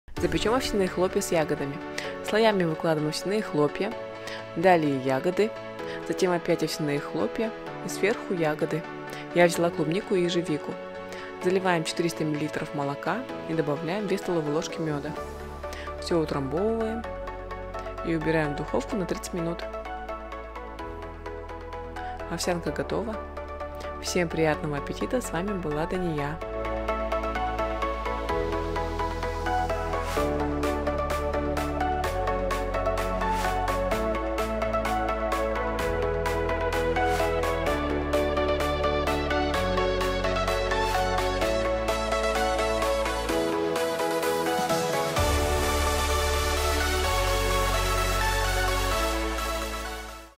Овсяные хлопья с ягодами в духовке | Запеченные овсяные хлопья с молоком
Надоела нам каша, нашла альтернативу - запечённые овсяные хлопья с ягодами🍓

Ставьте оценку рецепту от 1 до 5 👇

Ингредиенты:
Овсяные хлопья - 200 г
Молоко - 400 мл
Ягоды - 100 г

Можно использовать любые фрукты/ягоды на свой вкус, в следующий раз попробую с бананом🍌

Если каша немного поднадоела, то попробуйте приготовить это блюдо на завтрак, не забудьте поделиться результатом и отметить хештегом готовитдания 😋😊

PS. Всем хорошего дня 😊